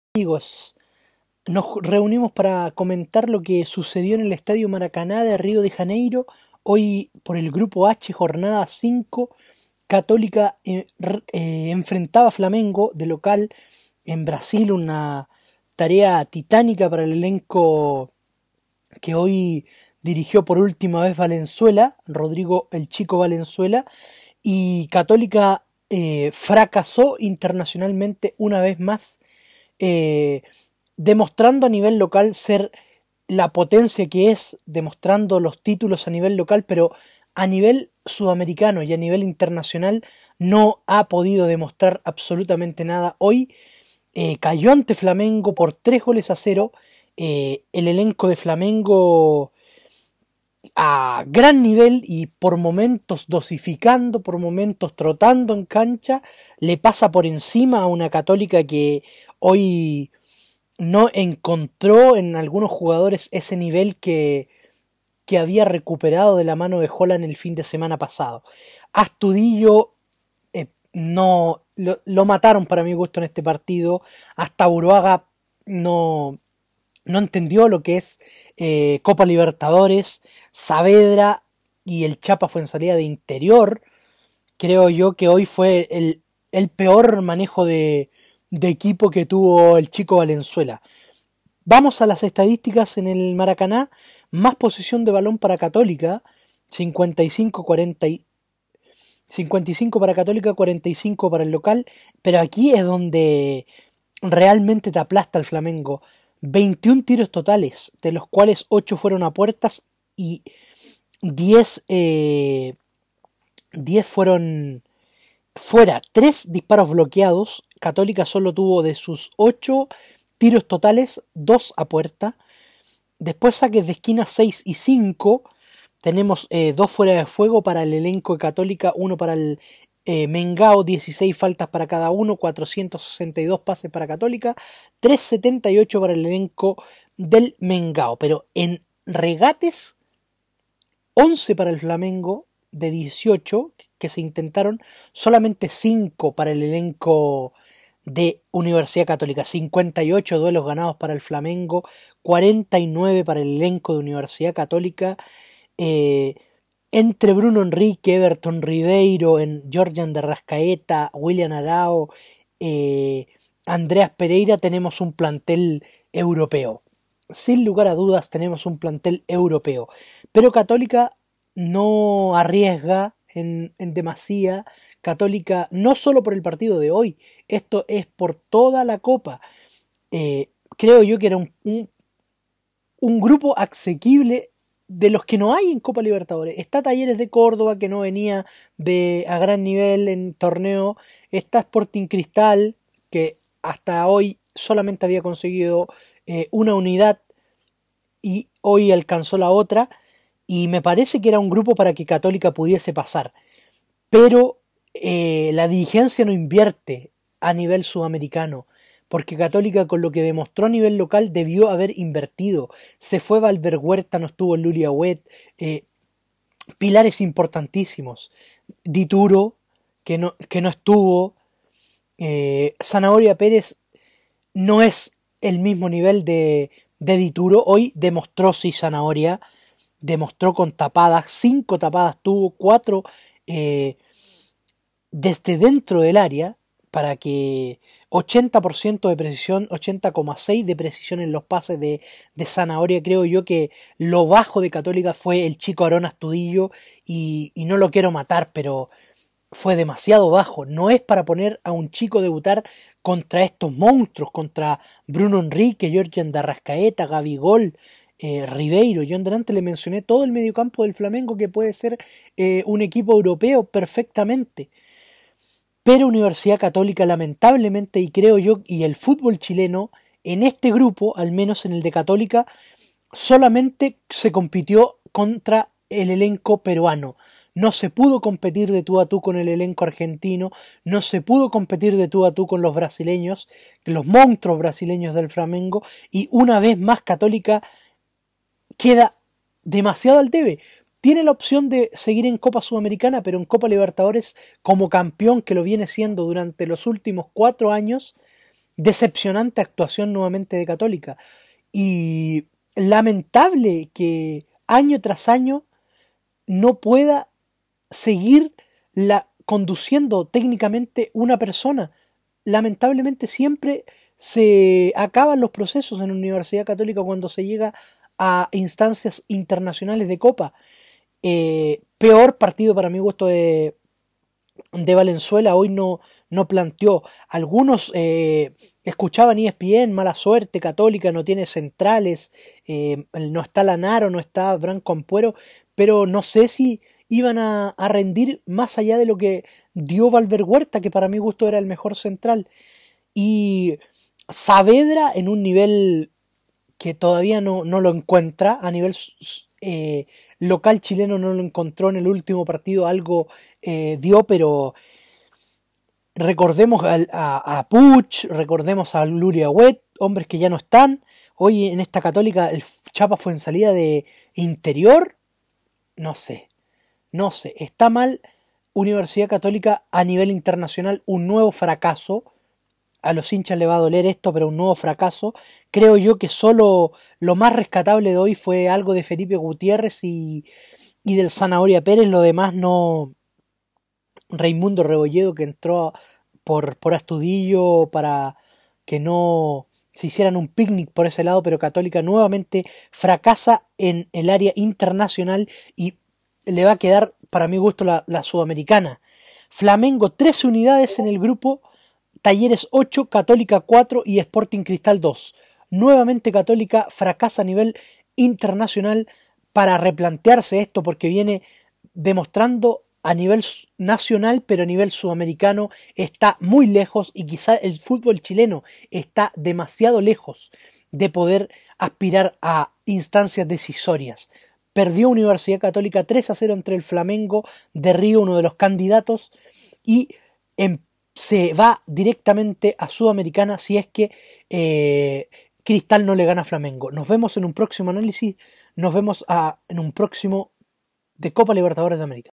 FLAMENGO 3 0 UCATÓLICA ::: COPA LIBERTADORES
FLAMENGO DEMUESTRA SU SUPREMACIA Y DERROTA CON TRANQUILIDAD 3 X 0 A UNIVERSIDAD CATOLICA QUE SE DESPIDE DEL TORNEO CON UN NUEVO FRACASO A NIVEL INTERNACIONAL DONDE NO PUEDE LAMENTABLEMENTE IGUALAR QUE LO VIENE REALIZANDO A NIVEL LOCAL

EL SHOW DEL ANALISIS DEL FUTBOL

    ^ MUNDOMANUMAN ^

PARTIDO  DE COPA A GRAN NIVEL EN SAN CARLOS DE APOQUINDO DONDE LA VISTA SE LLEVA LOS TRES PUNTO A RIO DE JANEIRO TRES GOLES A DOS